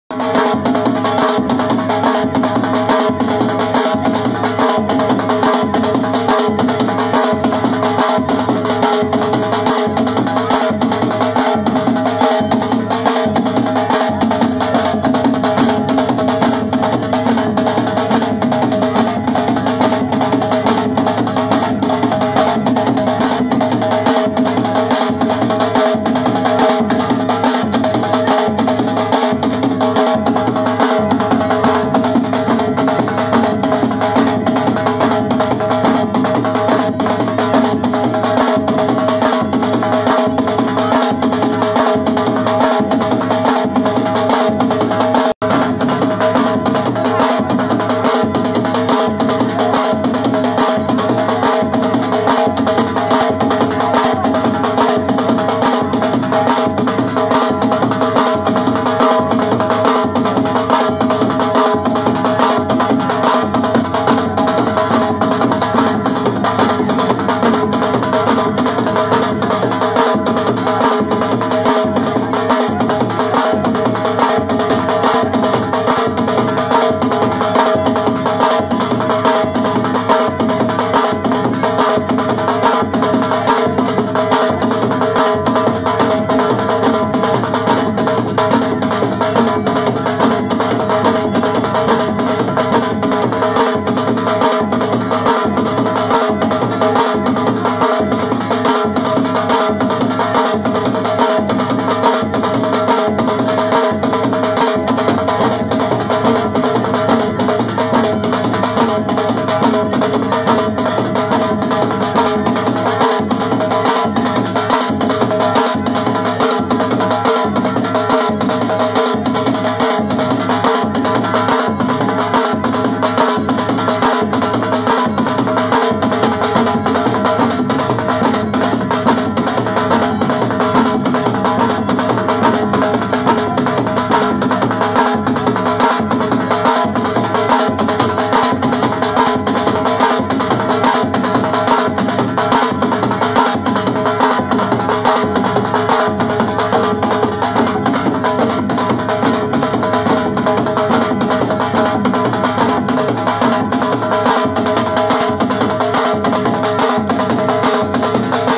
New kishtwar dance tagood : Folk Dances of India
kishtwar Dance Tagood : Folk Dances of India

Experience the vibrant culture of Kishtwar through this mesmerizing dance performance! This video showcases the traditional dance styles that are an integral part of our heritage.

Join us in celebrating the rich traditions of Kishtwar!